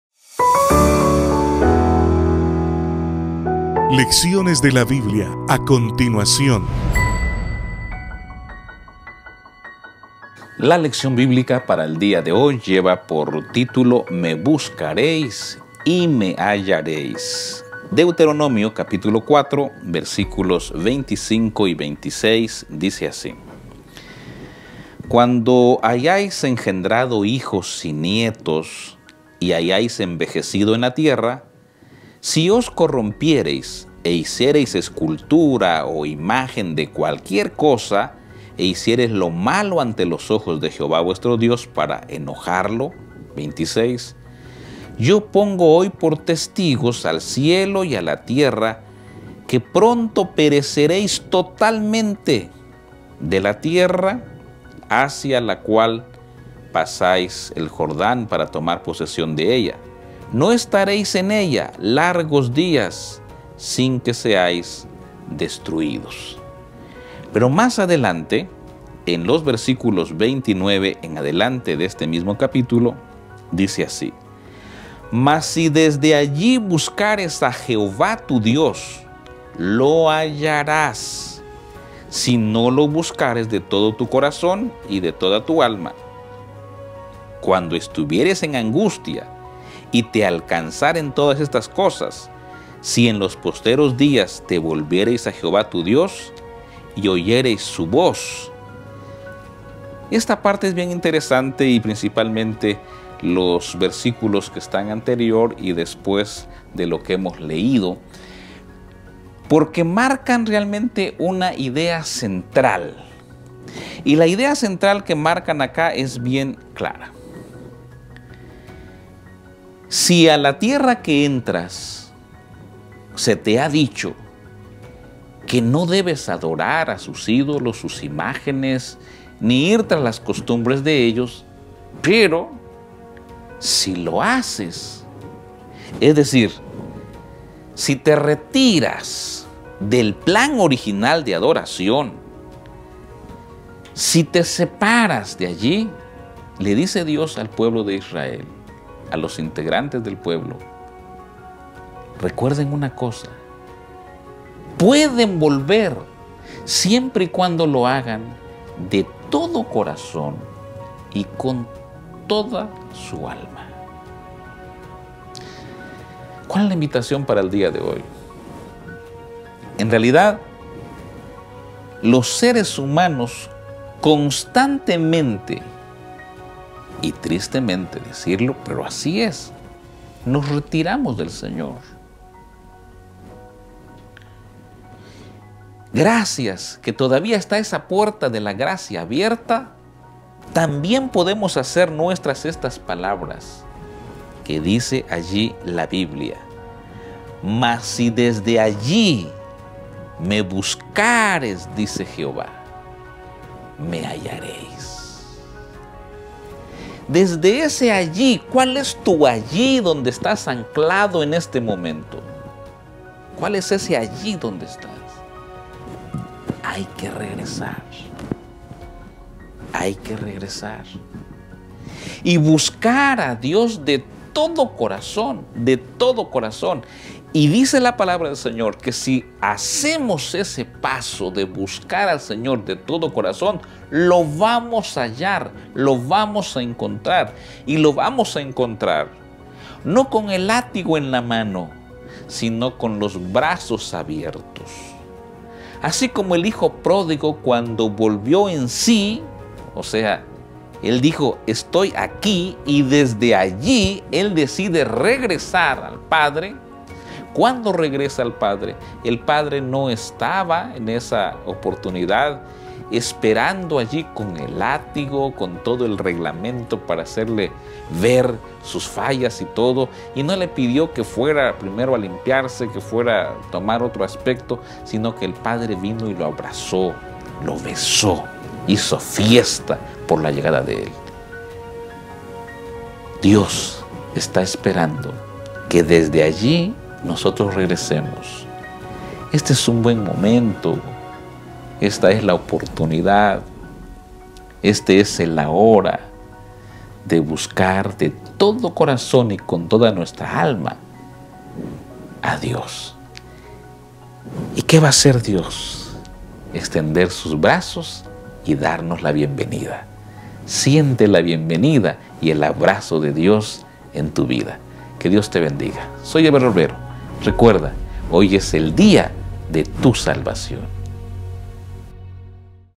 ME BUSCARÉIS Y ME HALLARÉIS - LDB 221121
La lección de este día se titula "Me buscaréis y me hallaréis", una vez más en los labios del Pr. Ever Roblero.